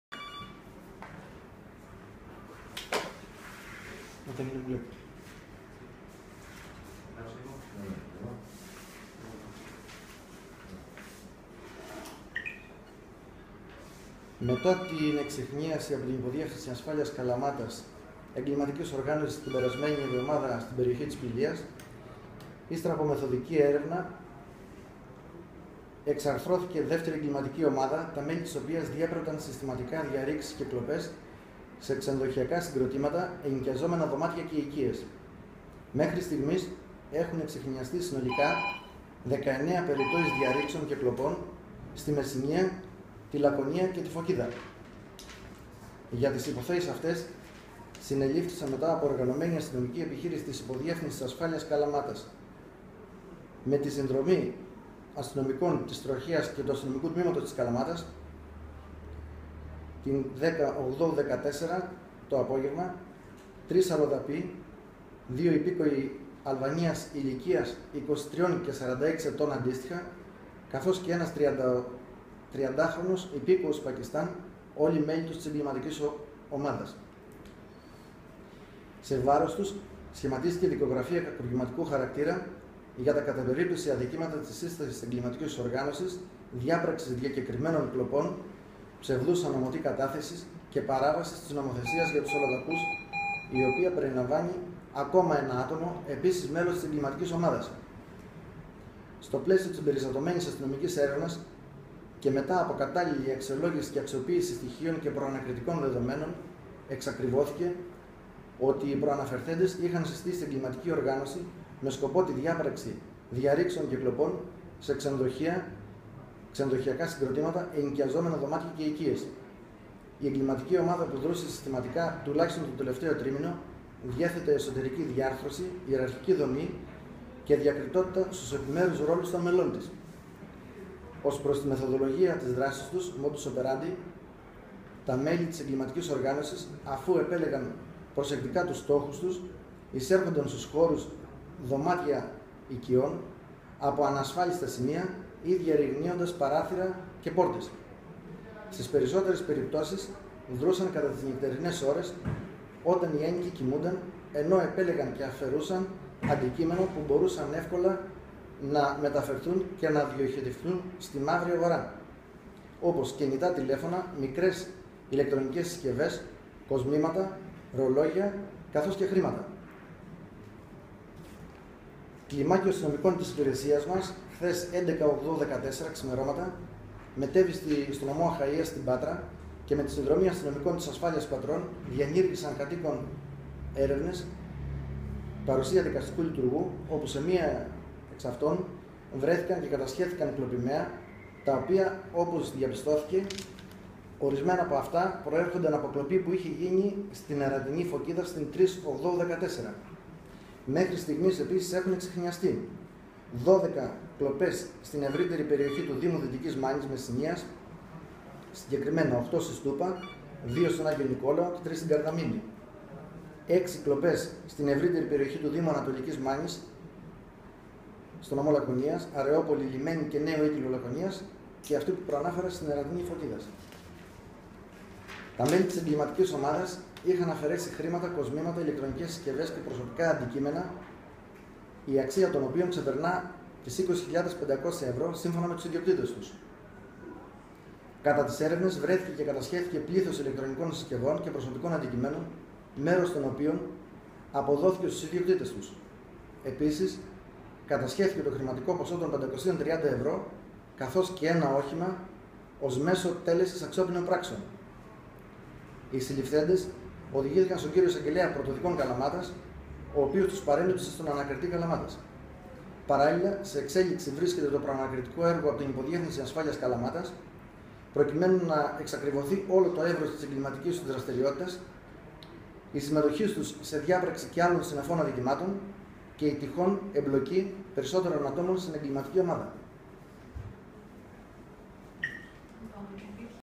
12-08-2014: Εξάρθρωση ομάδας που διέπραττε κλοπές σε ξενοδοχεία και ενοικιαζόμενα δωμάτια
Eξαρθρώθηκε εγκληματική ομάδα που διέπραττε συστηματικά κλοπές σε ξενοδοχειακά συγκροτήματα, ενοικιαζόμενα δωμάτια και οικίες στη Μεσσηνία, τη Λακωνία και τη Φωκίδα